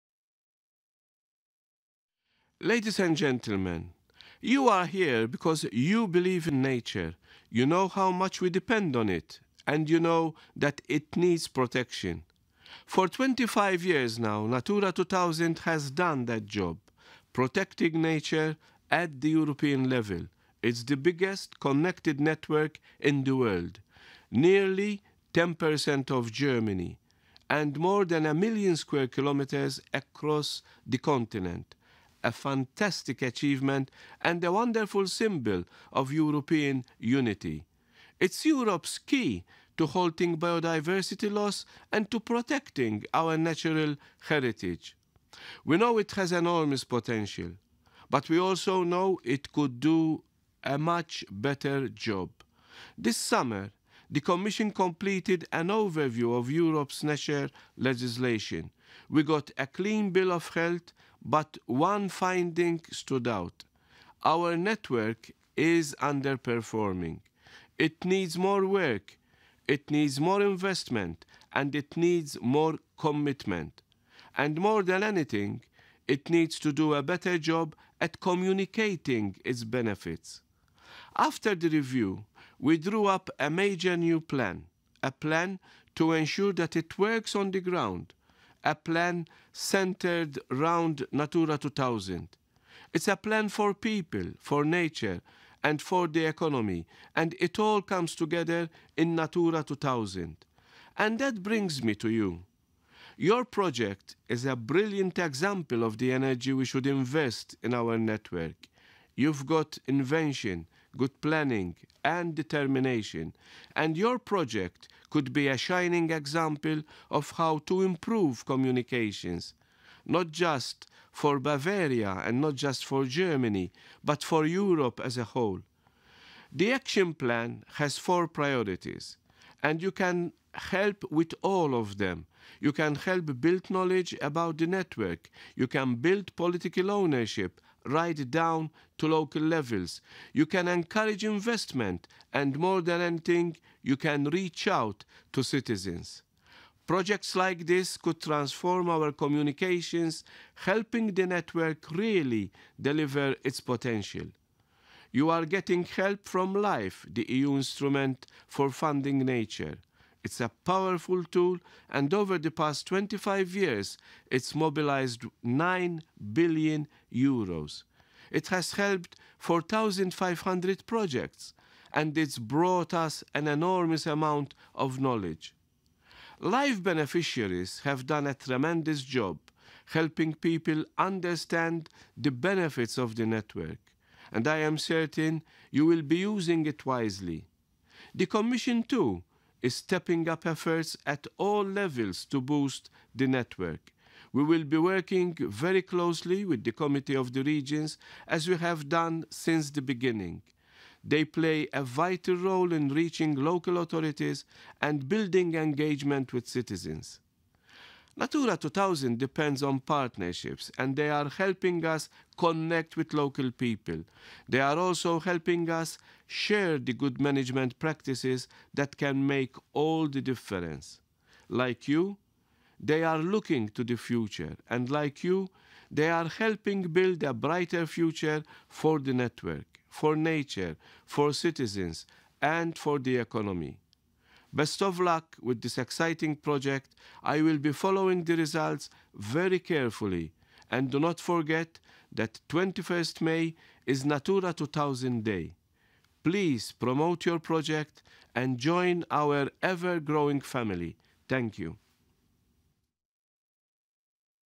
Karmenu Vella Video Opener
Videobotschaft von Karmenu Vella, dem EU Kommissar für Umwelt, Maritime Angelegenheiten und Fischerei,  zur Veranstaltungseröffnung auf dem 1. Bayerischen "Natura 2000"-Gipfel im Schloss Nymphenburg am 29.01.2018